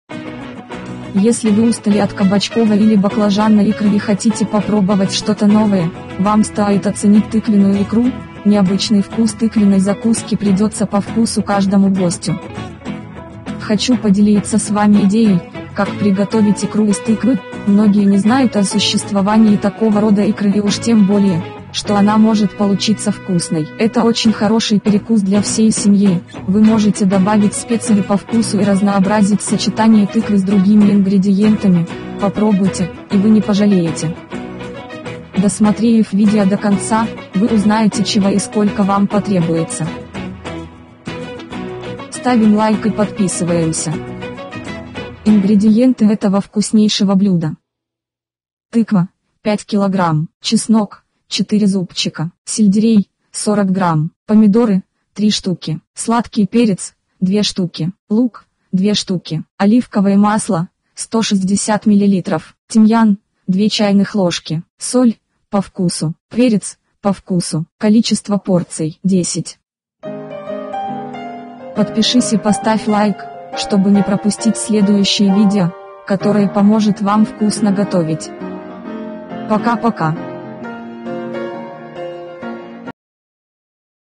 ★НЕРЕАЛЬНЫЙ ВКУС!★ ИКРА ИЗ ТЫКВЫ ★
★Новая инерпретация● Если вы устали от кабачковой или баклажанной икры и хотите попробовать что-то новое, вам стоит оценить тыквенную икру● Необычный вкус тыквенной закуски придется по вкусу каждому гостю●